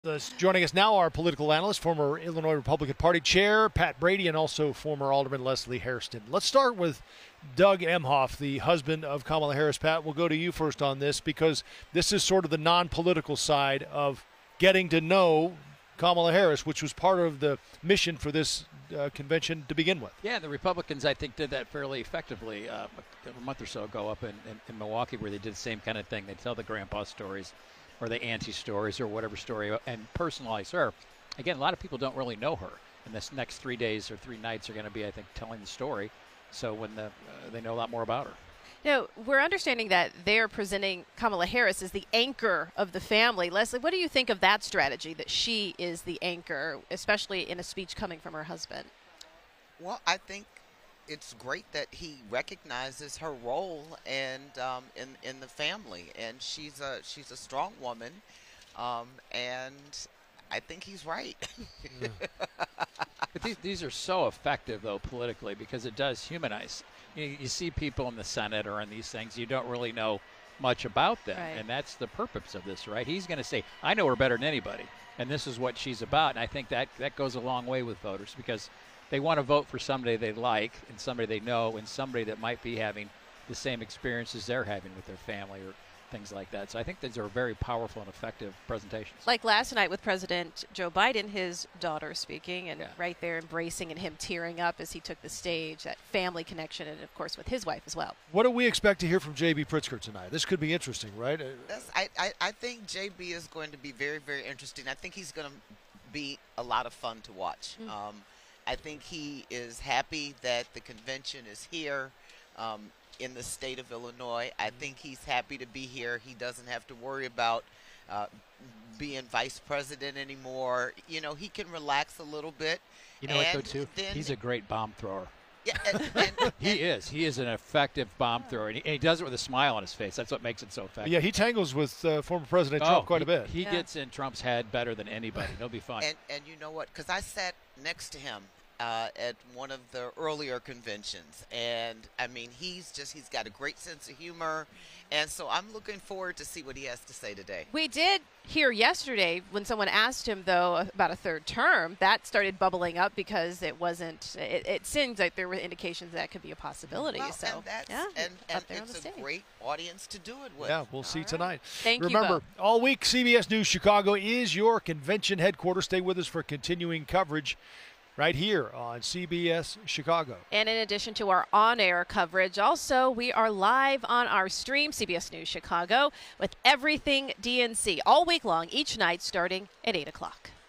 What to expect from the speeches on Night 2 of the DNC
CBS News Chicago political analysts Leslie Hairston and Pat Brady have a preview of what to expect at the Democratic National Convention on the second night—which will feature speeches by Illinois Gov. JB Pritzker, Second Gentleman Douglas Emhoff, and former President Barack Obama.